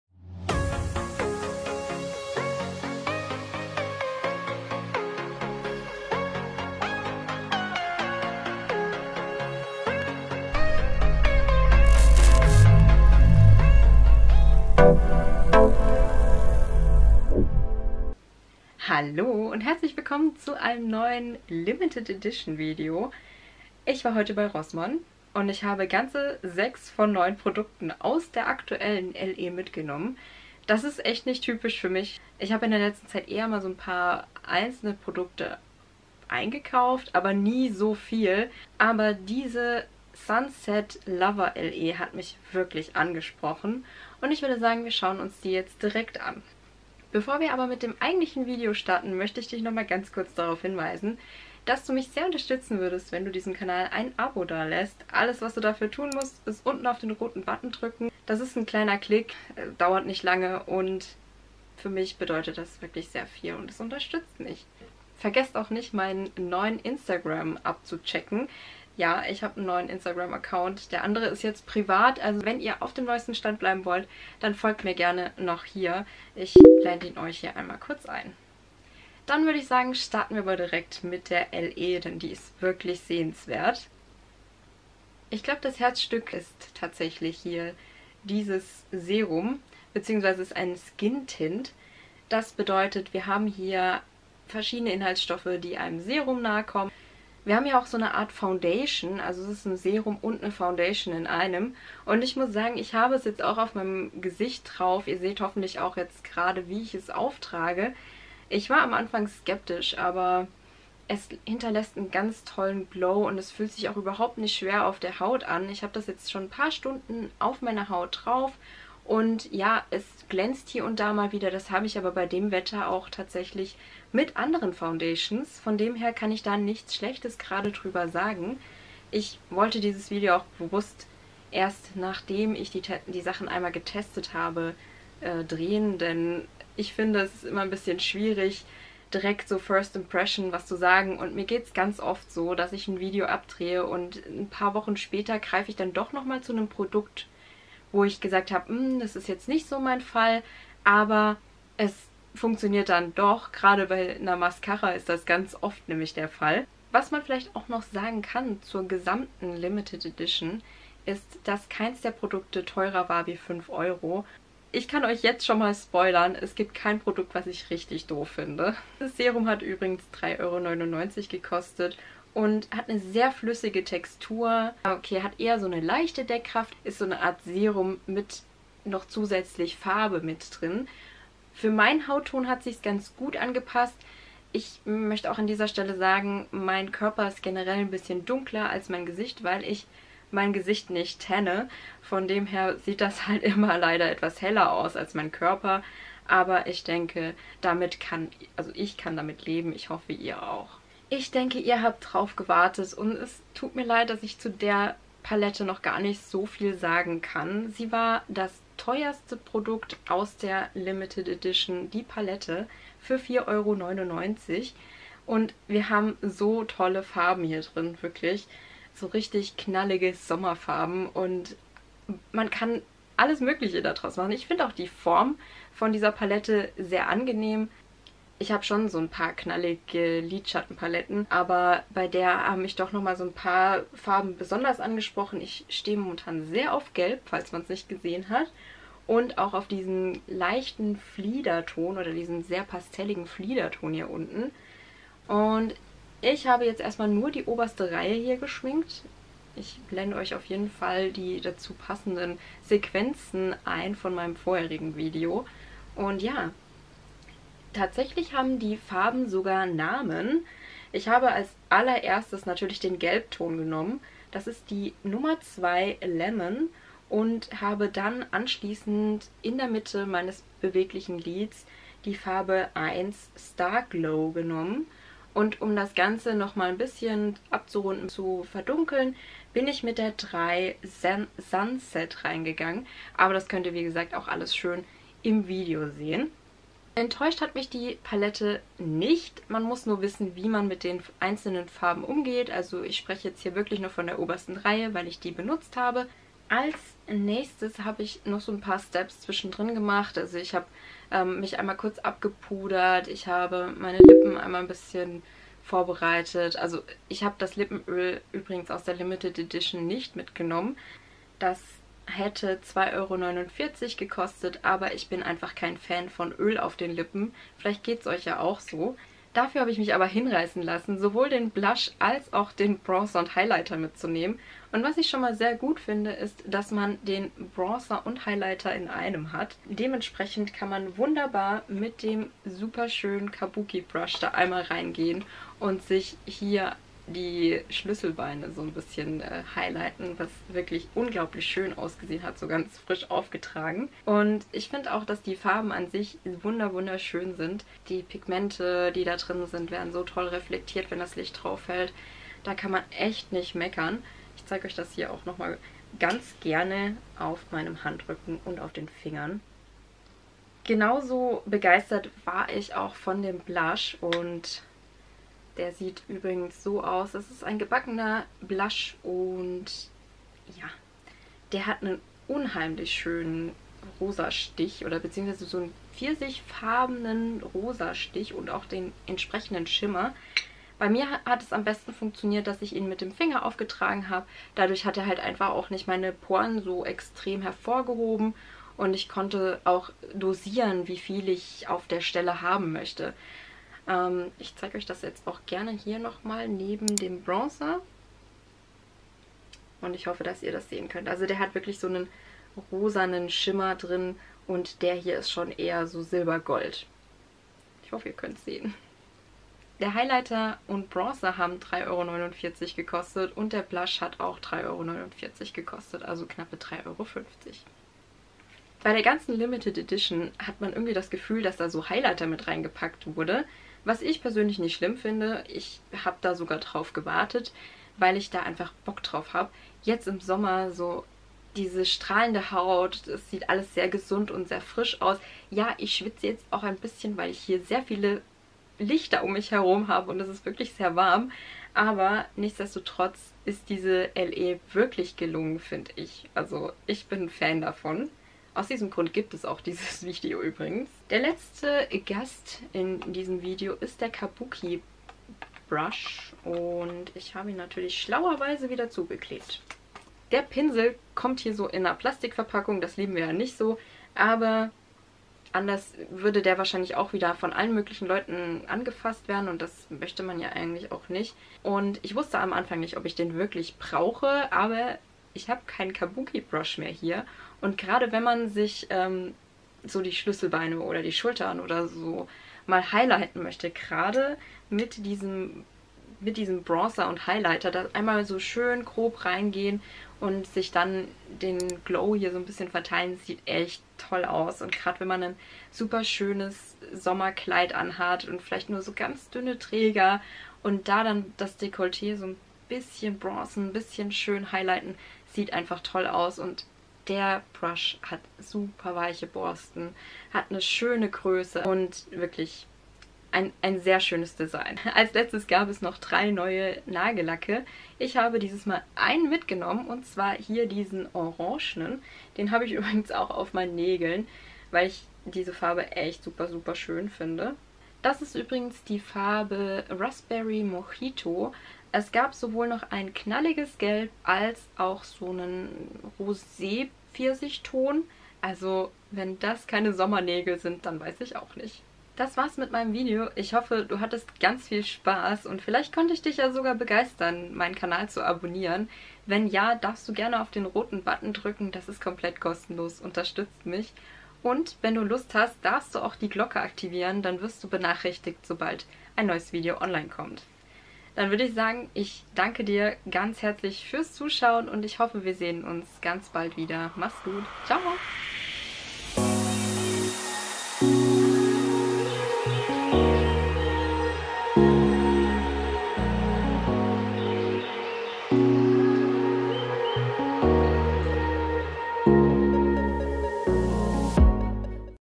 SUNSET LOVER | Rival de Loop | Sommer Limited Edition
Werbung | Anzeige wegen Markennennung

Bei Rossmann gibt es ganz neu (August 2022) die Sommer Limited Edition "Sunset Lover" von Rival de Loop! Ich habe für euch die Produkte getestet und berichte über meine Erfahrung.

Willkommen in der Flamazing Family !

Du bist noch kein Mitglied?

Fragen, Feedback oder Wünsche? - Ab in die Kommentare